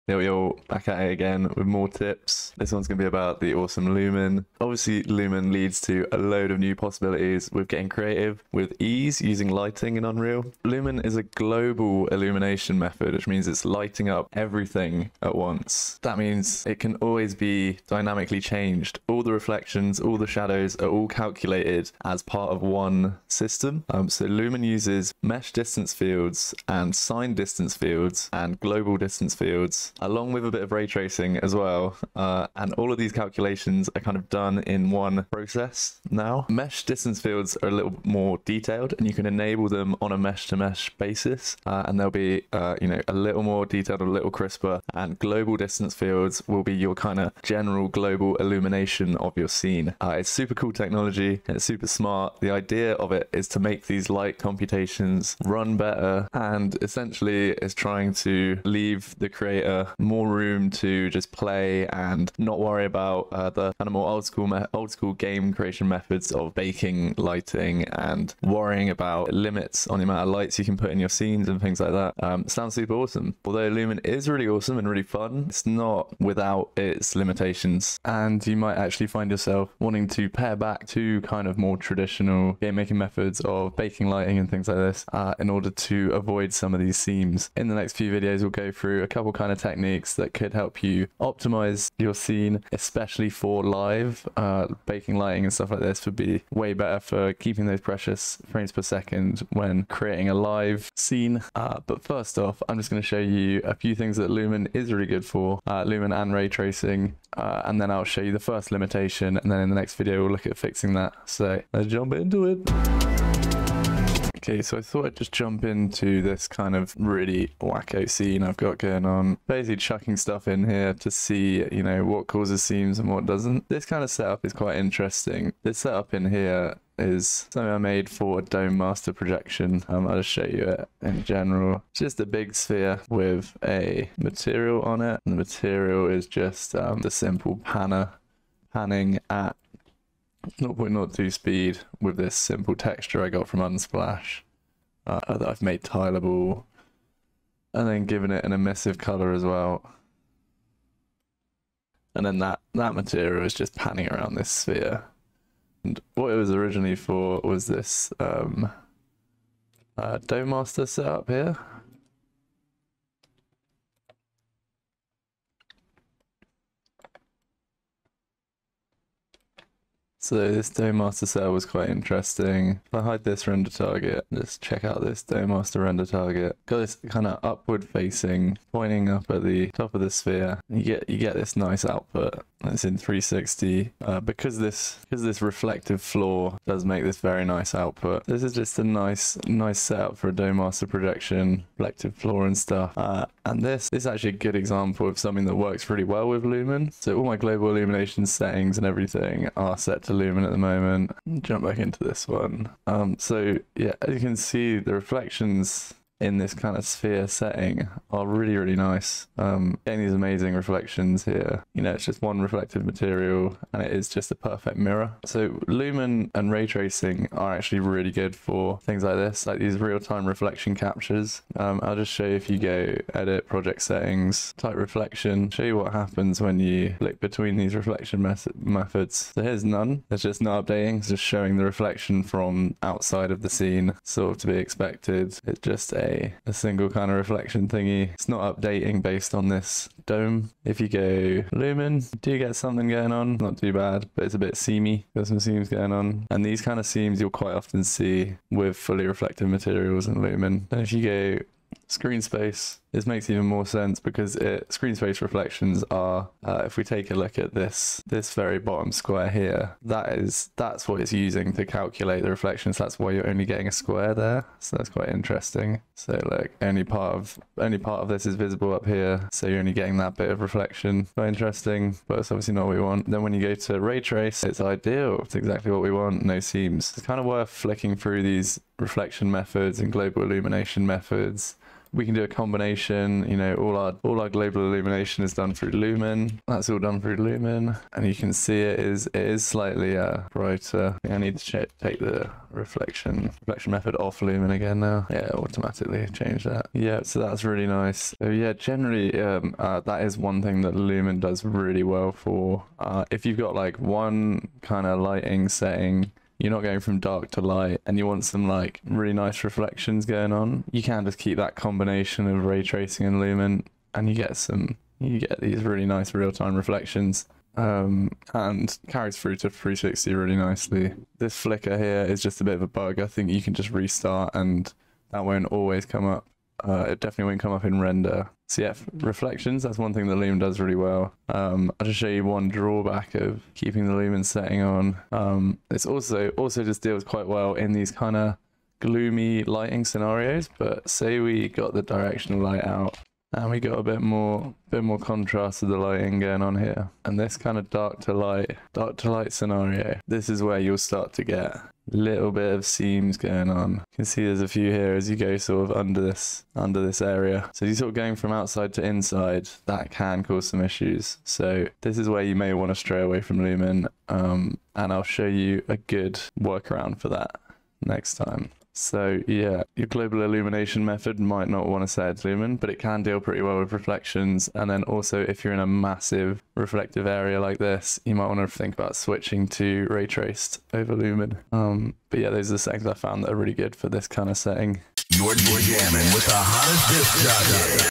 How to use LUMEN in 360 degree renders, avoiding seams and artefacts in Unreal Engine
Lumen is awesome, but can be a pain sometimes. It's new technology and isn't always super compatible. Here's where I've found Lumen useful and where it's been problematic for 360 renders in Unreal Engine.

𝘽𝙊𝙒𝙏𝙄: (𝘽𝙀𝘾𝙊𝙈𝙀.𝙊𝙉𝙀.𝙒𝙄𝙏𝙃.𝙏𝙃𝙀.𝙄𝙉𝙏𝙀𝙍𝙉𝙀𝙏)

If these videos have made you want to buy the full license to the toolkit then please use this link!